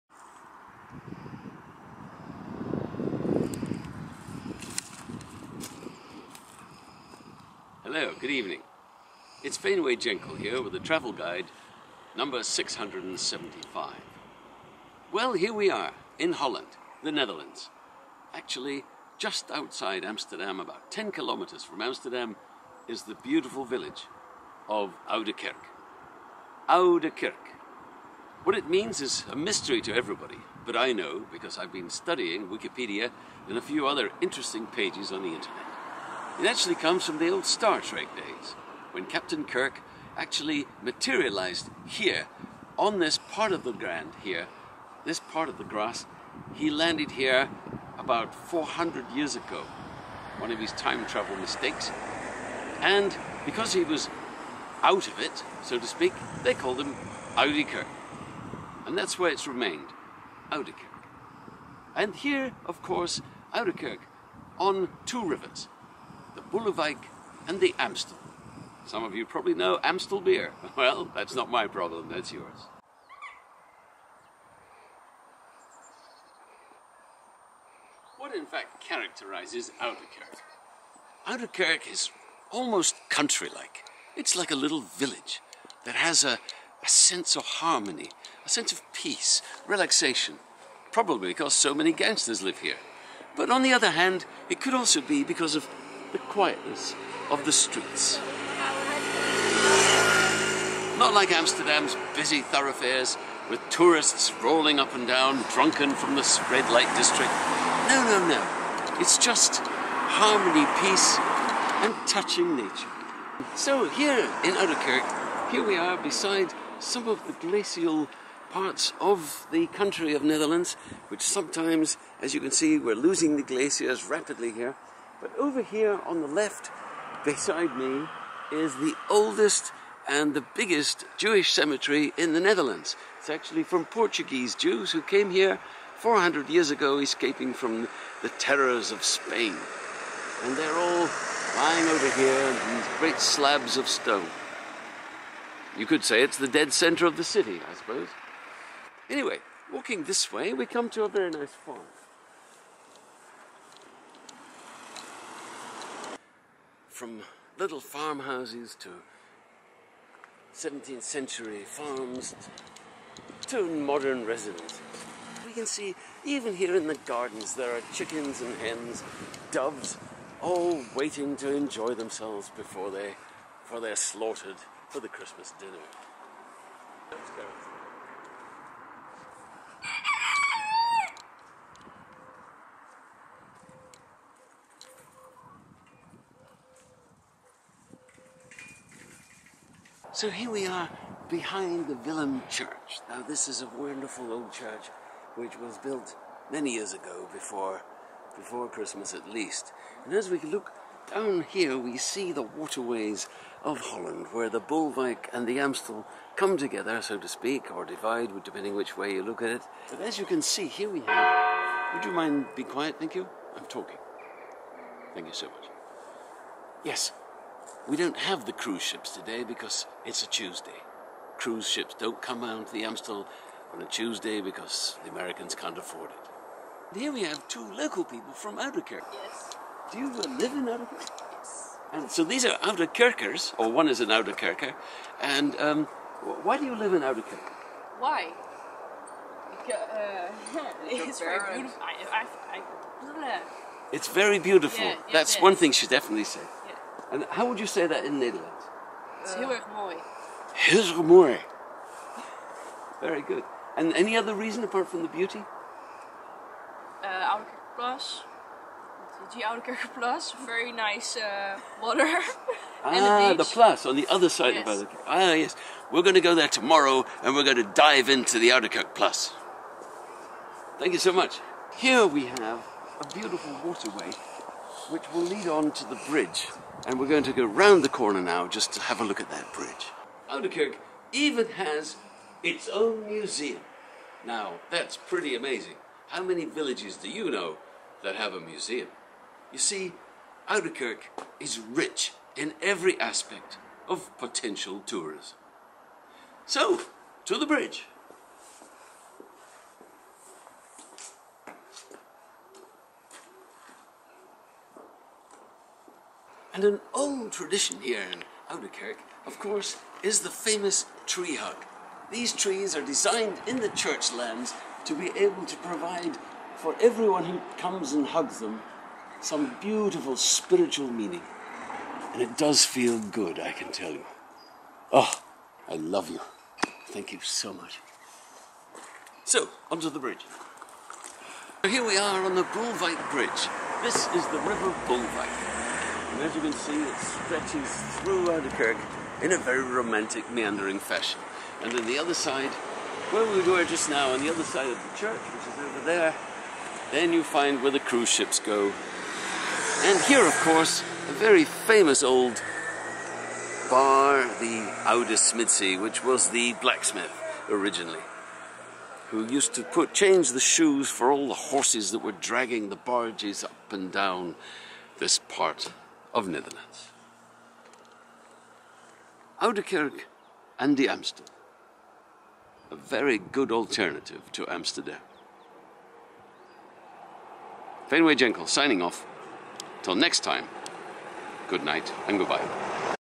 Faynway Jenkell travel guide no.675 Oudekerk aan de Amstel
The illustrious Faynway Jenkell with his travel guide no.  on the specially magical village on the edge of Amsterdam